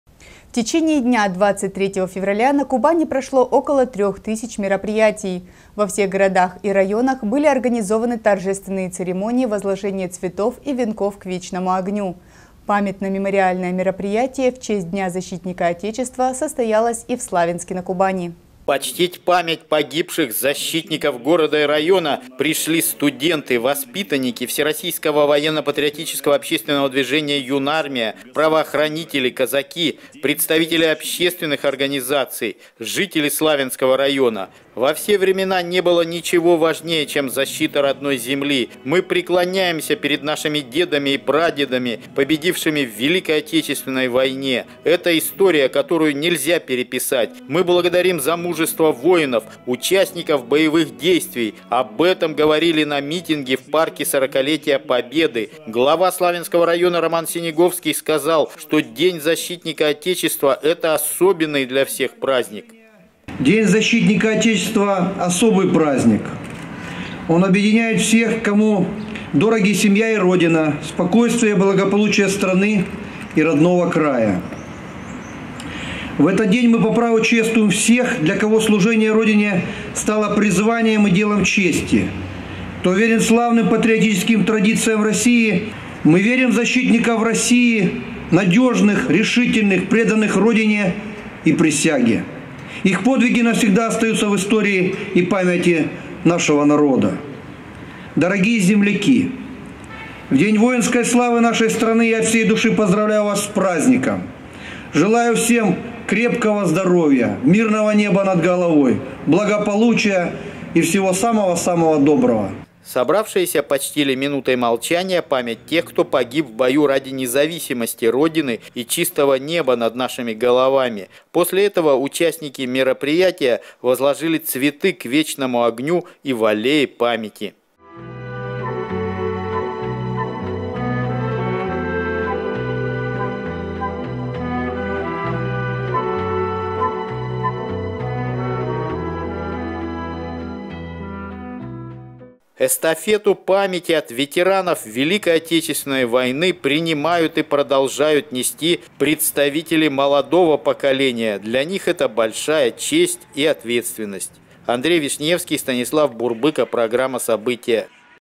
Верность долгу и служение Родине
В течение дня 23 февраля на Кубани прошло около 3 тысяч мероприятий. Во всех городах и районах были организованы торжественные церемонии возложения цветов и венков к Вечному огню. Памятно-мемориальное мероприятие в честь Дня защитника Отечества состоялось и в Славянске - на - Кубани